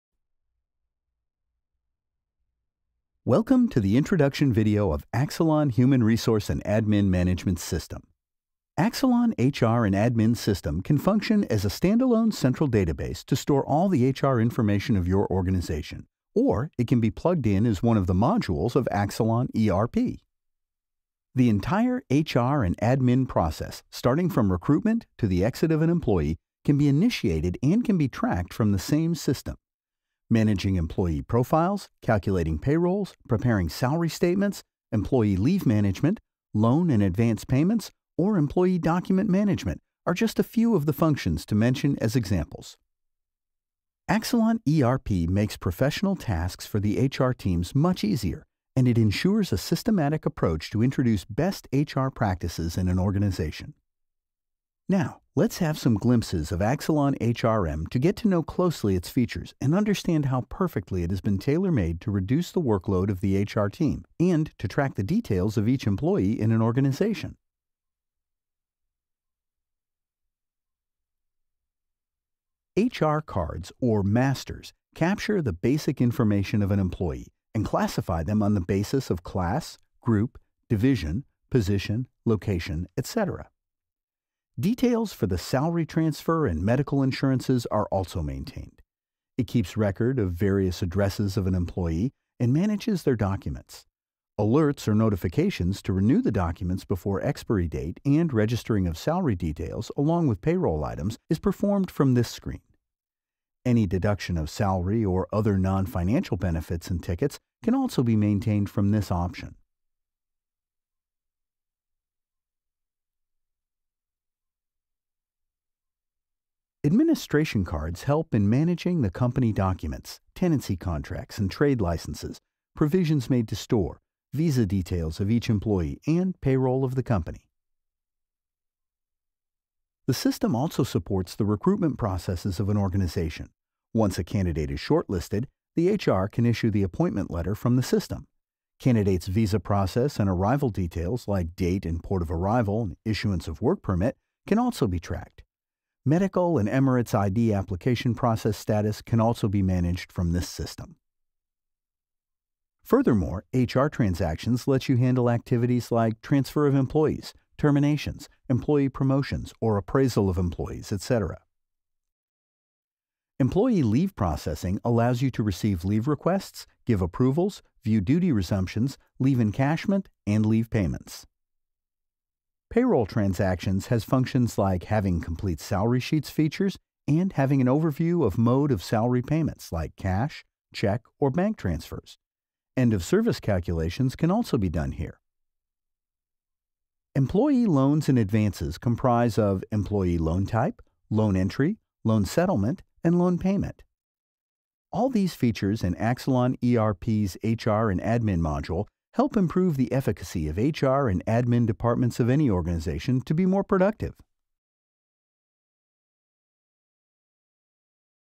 Axolon ERP HR & Admin Modules
This video overviews the HR & Admin Modules - Demo

Axolon ERP helps you manage your employees and administration easily by using HR and Payroll Software for small and mid-sized business and with minimum effort. You can track all your employee activities and track the full history of employment with a single click.

Axolon ERP helps the HR team to calculate the salary and overtime of each employee. It also manages documents like visa, passport & DL and monitors the benefits & liabilities each employee hold in the organization.